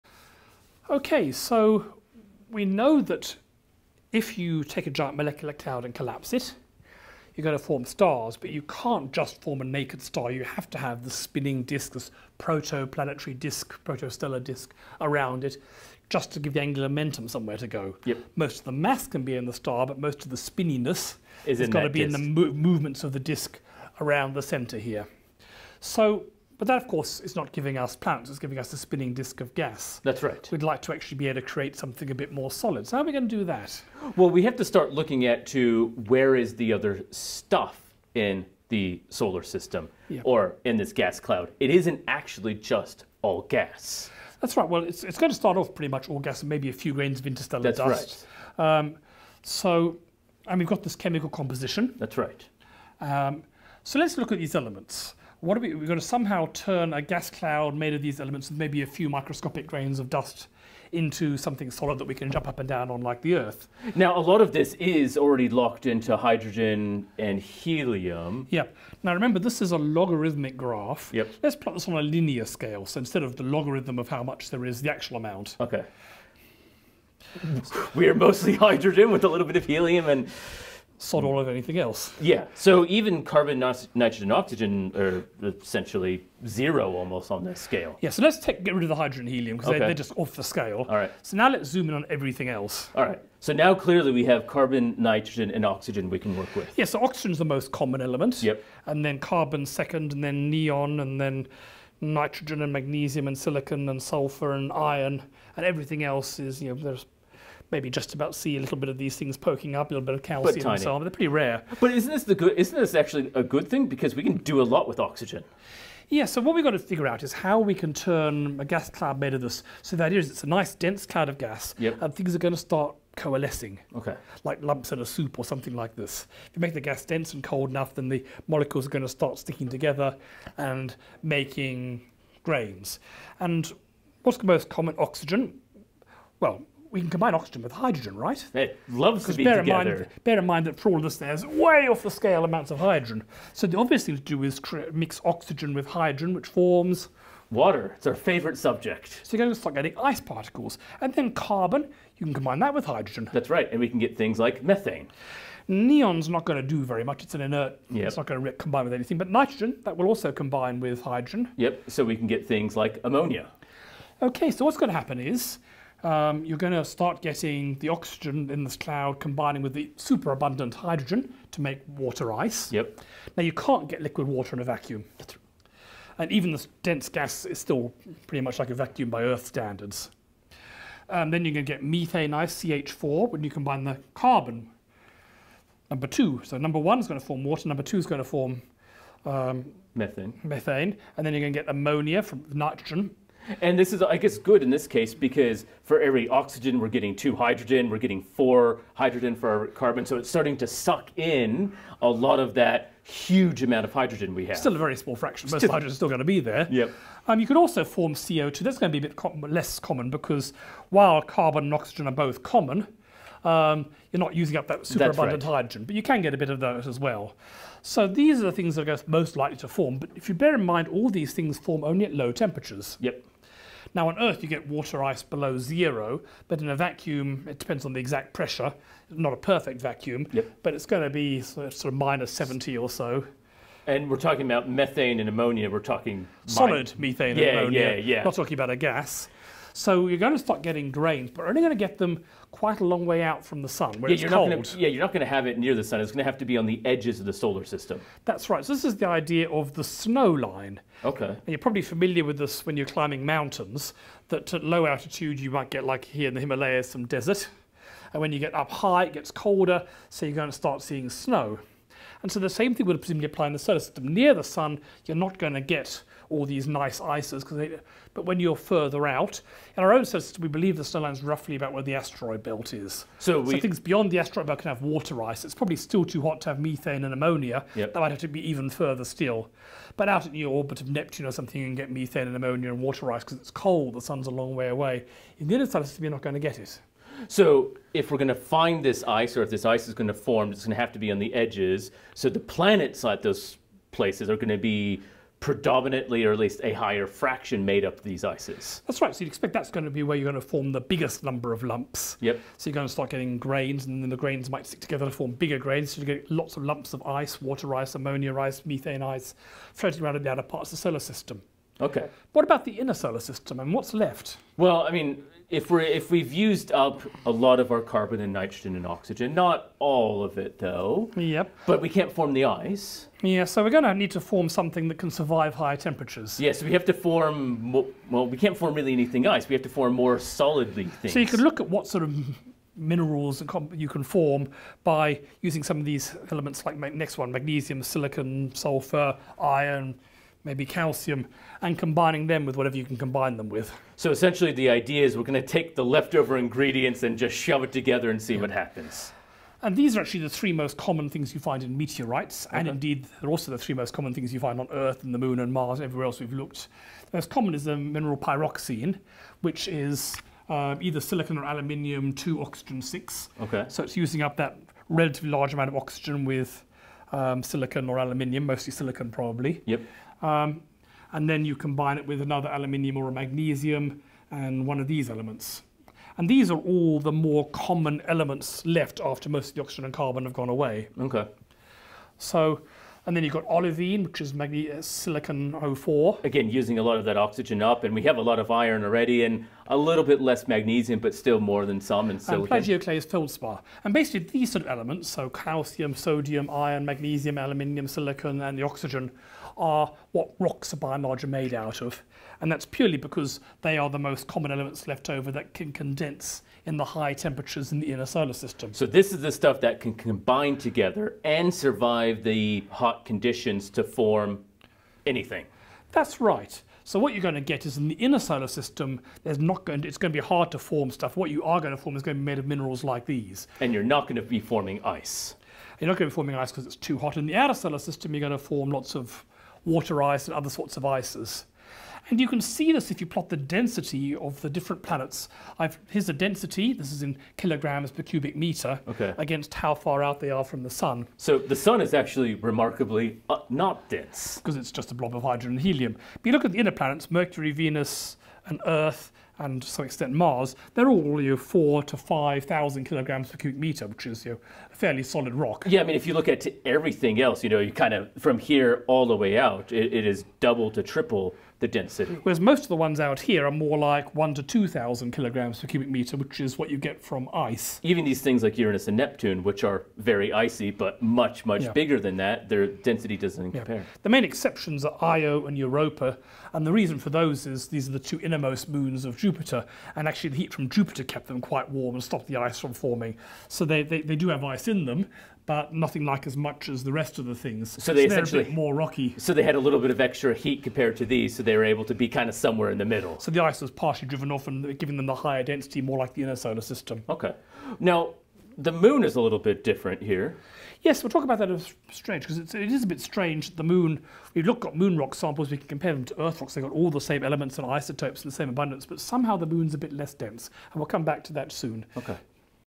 Planets V3.6 Forming Grains
The formation of solid grains in proto-planetary disks,  the elements they are made of and the snow-line.